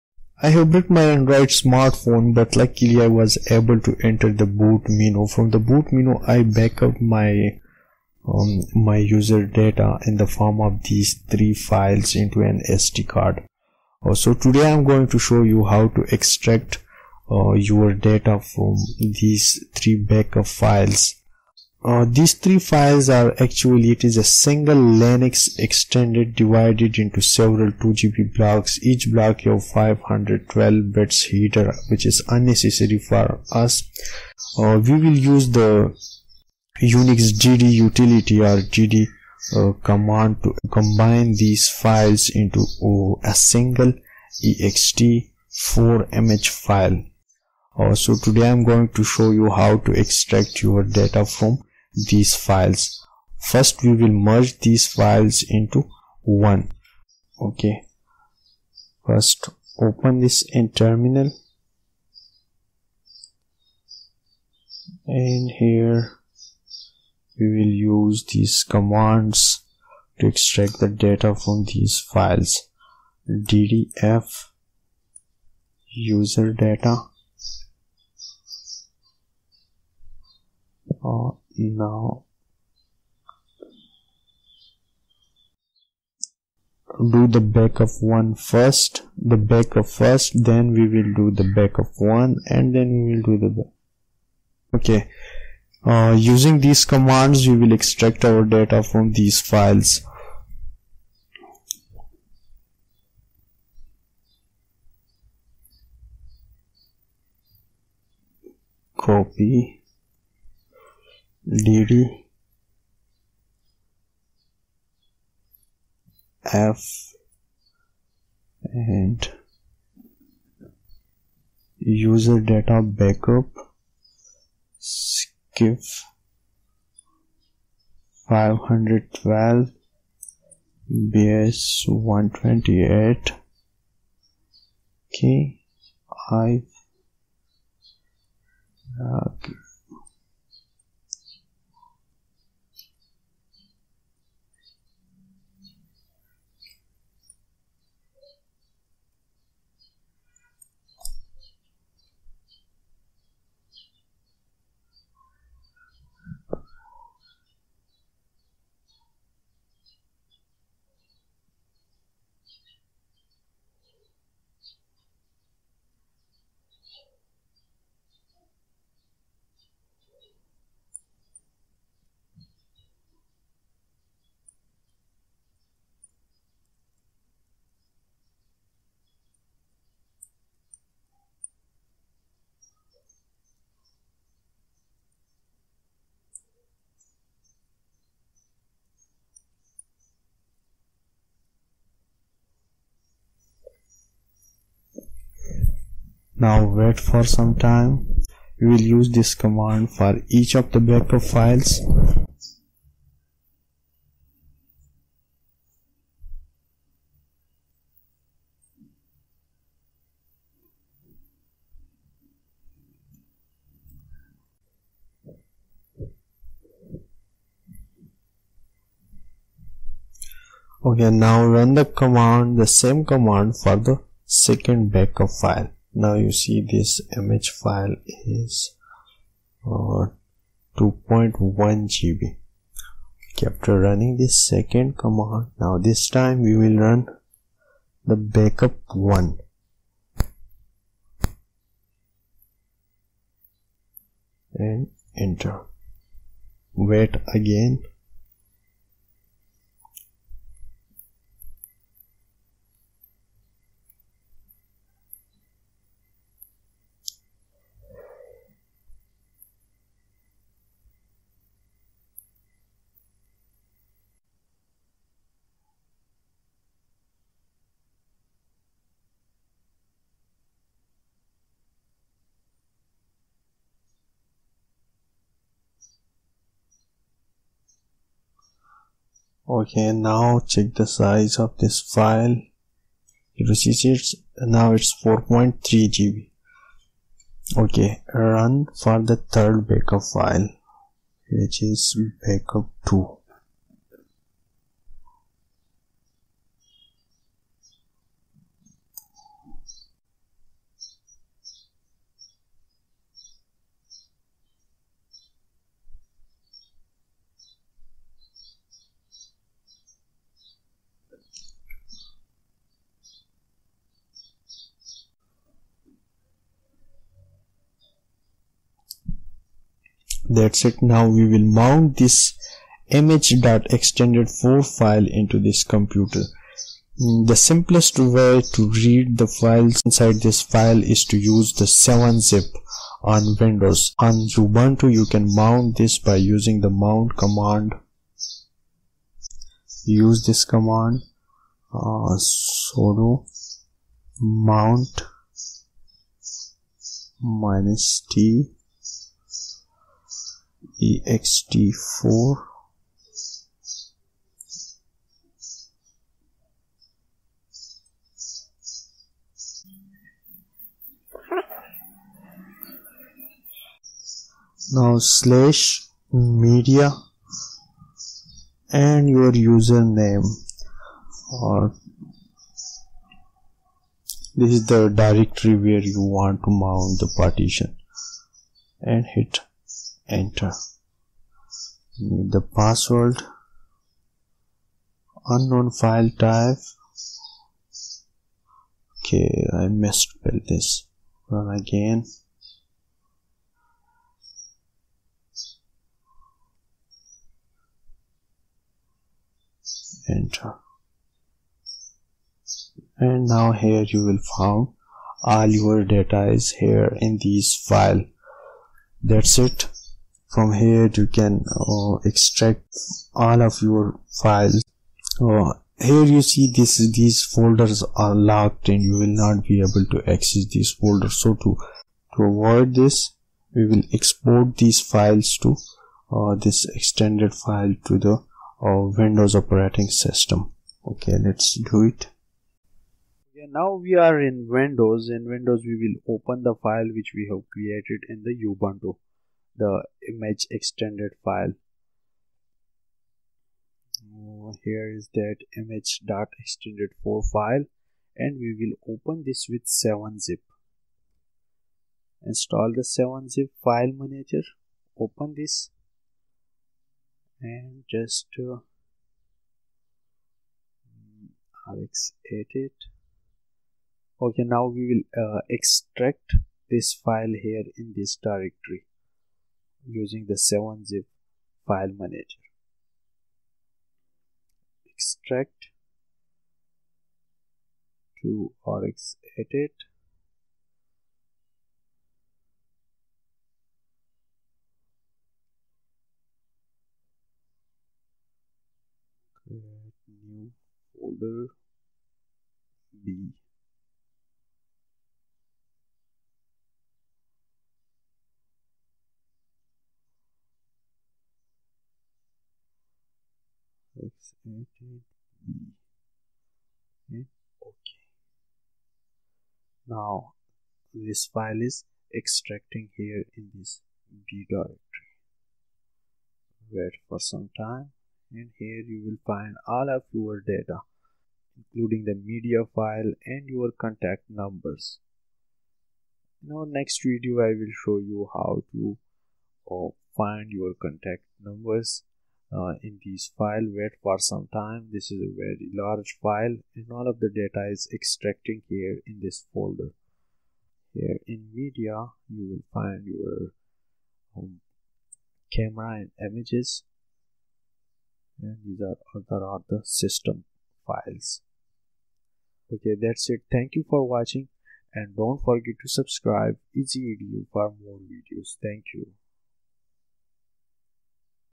How to extract data from .backup files created by android system backup on pc Ubuntu
I have finally recovered all of my data from my broken android smart phone, the only option i was left was to access the boot menu of my phone in the boot menu i backed up all of my data to sd card in the form of three files name userdata_xxxxxxxx_xxxxxx.backup 1 and 2. finally i found a solution how to extract the data from these files watch the video and subcribe bellow the detail information of these files and the commands used to extract data from these files. on ubuntu (linux) and windows operating system
There were no exact information about their format. Appeared, that it is single Linux Ext4 divided into several 2Gb blocks. Each block has 512 byte header unnecessary for us. Run unix dd unility (or dd.exe under Windows, or even bbcopy.exe) to reconstruct Ext4 image in single file. Do it on partition supporting large files above 2Gb (NTFS, Linux Ext4, FBSD UFS, etc.)
dd if=userdata_20100101_000617.backup skip=512 bs=128k iflag=nocache,skip_bytes oflag=nocache,append conv=notrunc of=img.ext4
dd if=userdata_20100101_000617.backup1 skip=512 bs=128k iflag=nocache,skip_bytes oflag=nocache,append conv=notrunc of=img.ext4
dd if=userdata_20100101_000617.backup2 skip=512 bs=128k iflag=nocache,skip_bytes oflag=nocache,append conv=notrunc of=img.ext4
for mounting using ext4 file use the following command /mnt/xam is for the directory
mount -t ext4 -o loop img.ext4 /mnt/

you can open the ext4 file in 7zip on windows operating system